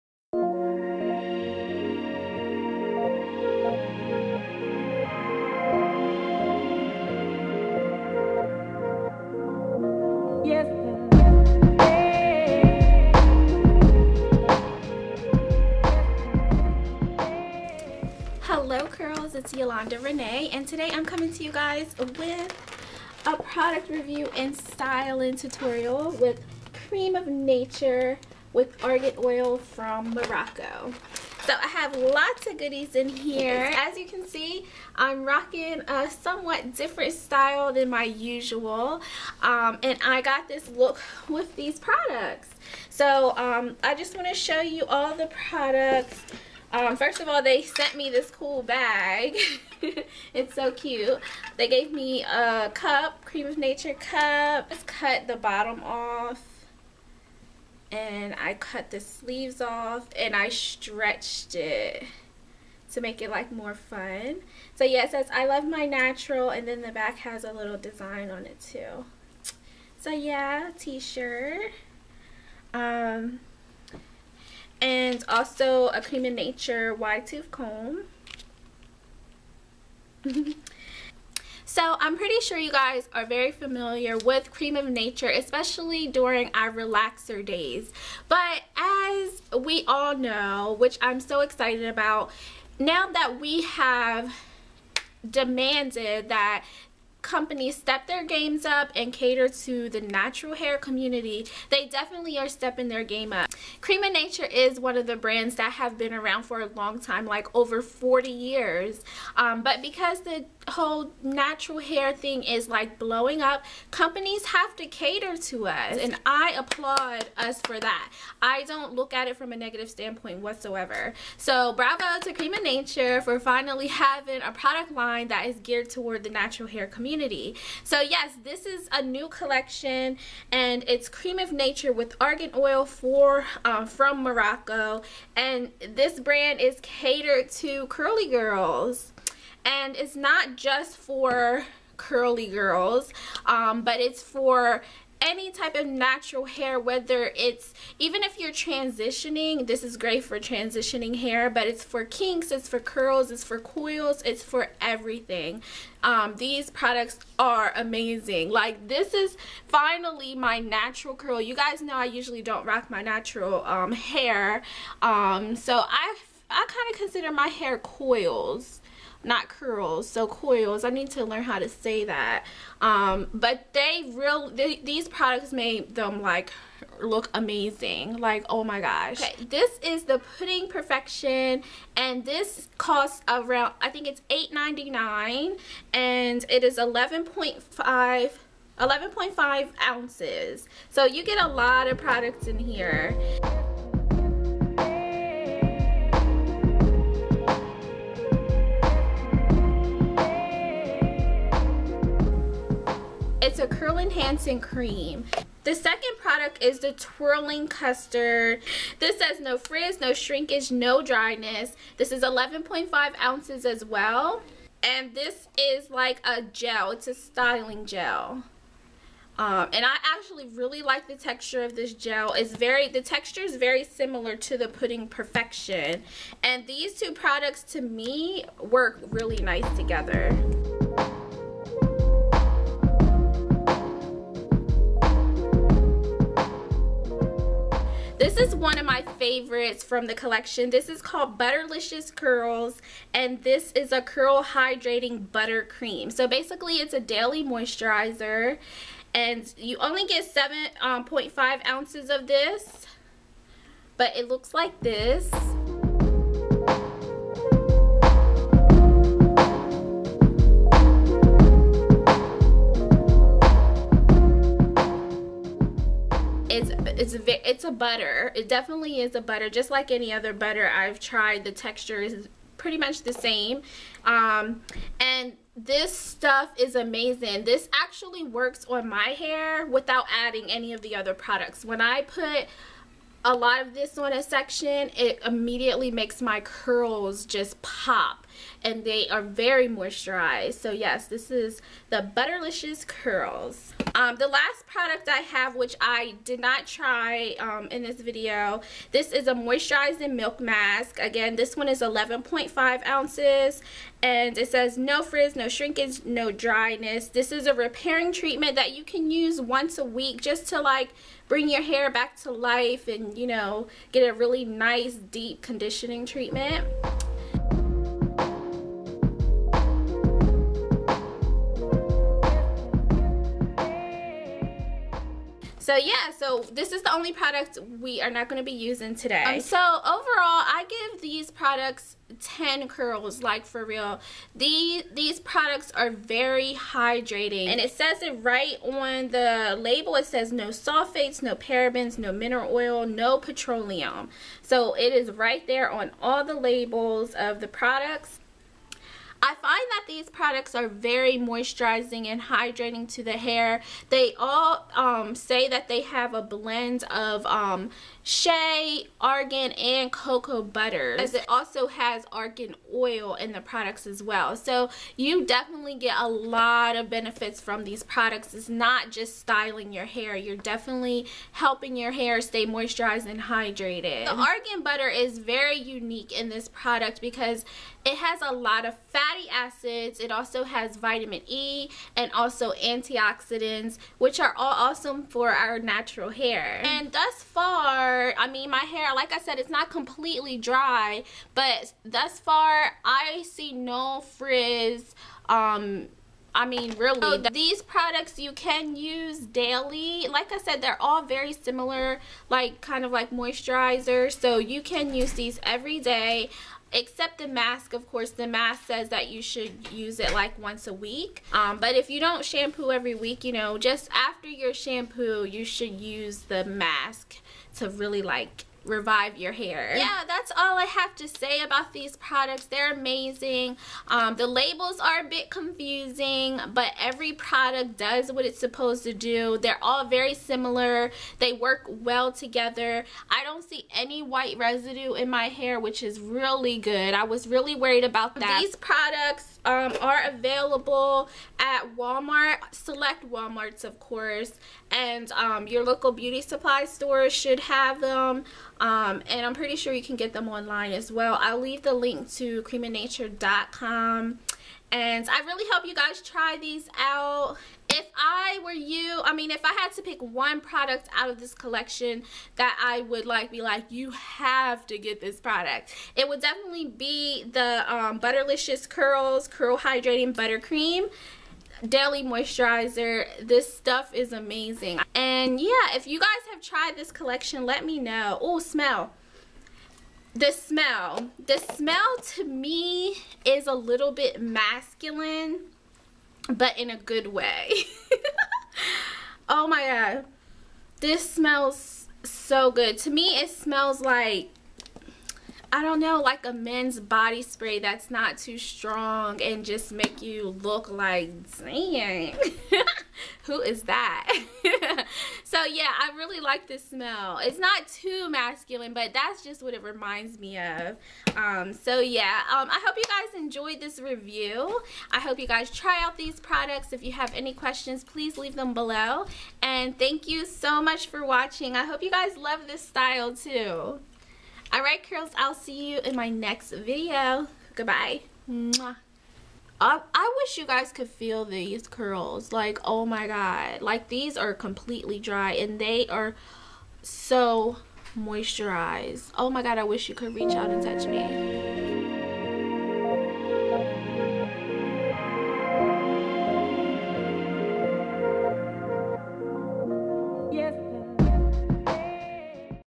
NATURAL HAIR | Creme of Nature with Argan Oil from Morocco Product Review
Special thanks to Crème of Nature for providing me with these products. This is my honest review! If you have any questions beyond what I mention, please leave them below.

I have permission for the use of the song/track in this video.

In this video I give you a product review of the Crème of Nature with Argan Oil from Morocco collection, You can get these products from your local Walmart and beauty supply stores. See how I styled my hair using these products,